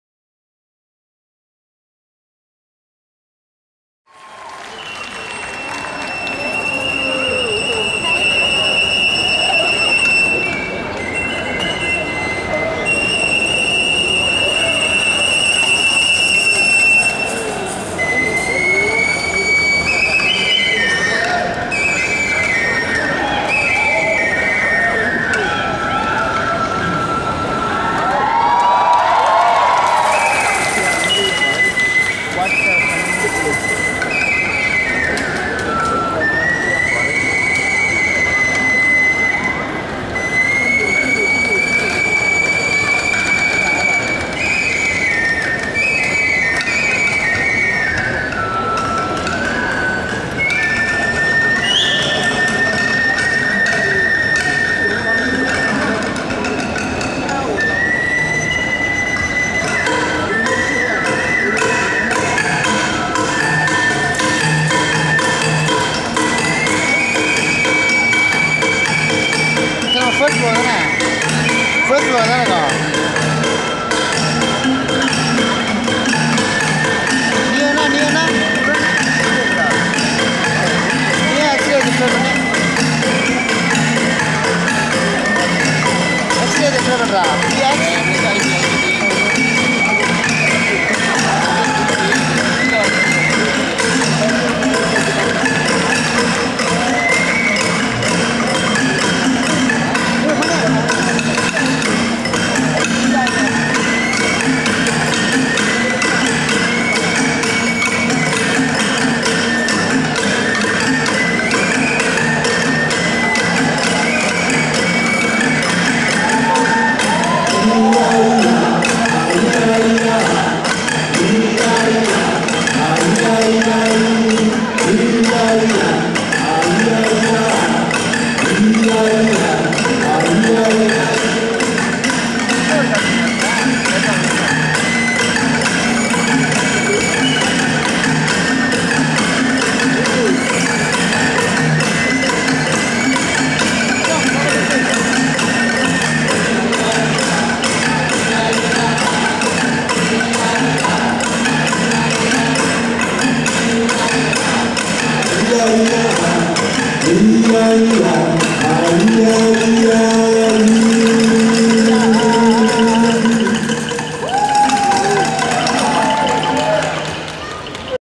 Bonita Group from Vietnam Performs at Royal Belum World Drums Festival 2012
The Bonita Group from Vietnam is one of the several international percussion bands that took part in a "pre-show" Royal Belum World Drums Festival 2012, held at the Taiping Sentral Mall on 21 February 2012.

The Bonita group, dressed in colourful costumes, provided a lively and fast beats using bamboo and drums and vowed the crowd with their gracious moves. The Royal Belum Forest in Perak, Malaysia, contains the largest contiguous expanse of unprotected forest in Peninsular Malaysia. It's one of the world's most biologically diverse regions and one of Asia's highest priority areas for biodiversity conservation.